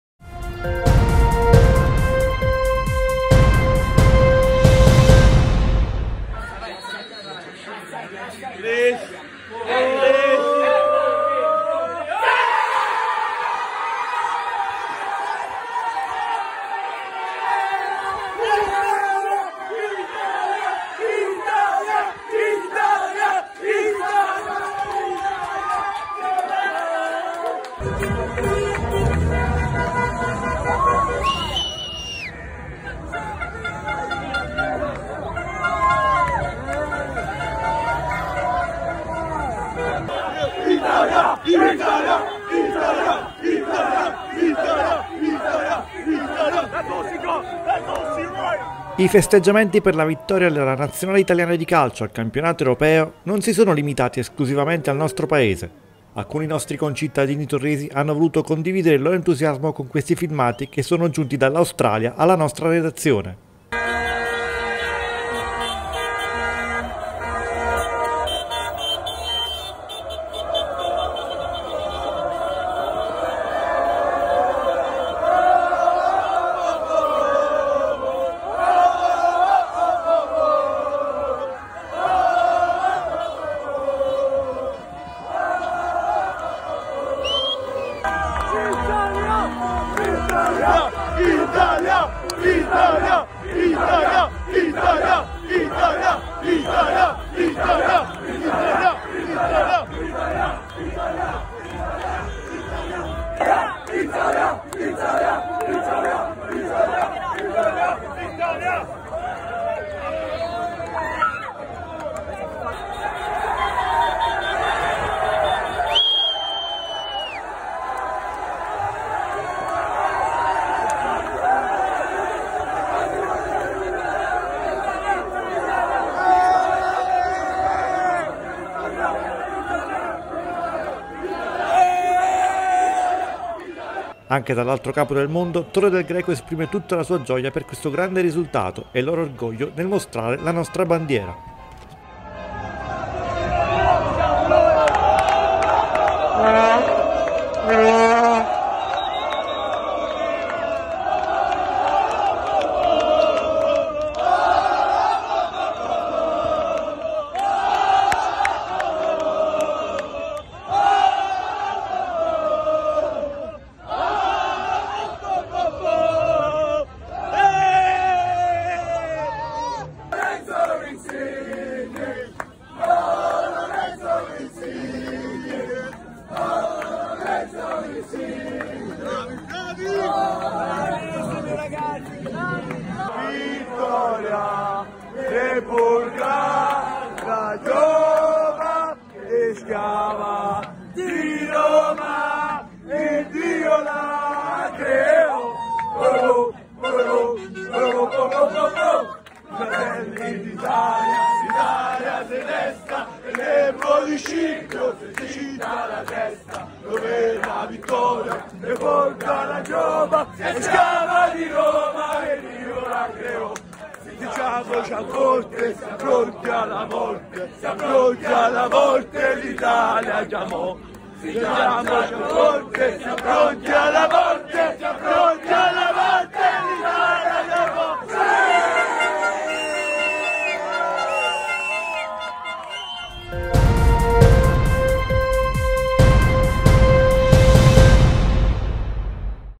Torresi in Australia festeggiano la vittoria del Campionato Europeo - VIDEO
I festeggiamenti per la vittoria della Nazionale Italiana di calcio al Campionato Europeo non si sono limitati esclusivamente al nostro Paese. Alcuni nostri concittadini torresi hanno voluto condividere il loro entusiasmo con questi filmati che sono giunti dall’Australia alla nostra redazione.
Anche dall’altro capo del mondo Torre del Greco esprime tutta la sua gioia per questo grande risultato e il loro orgoglio nel mostrare la nostra bandiera.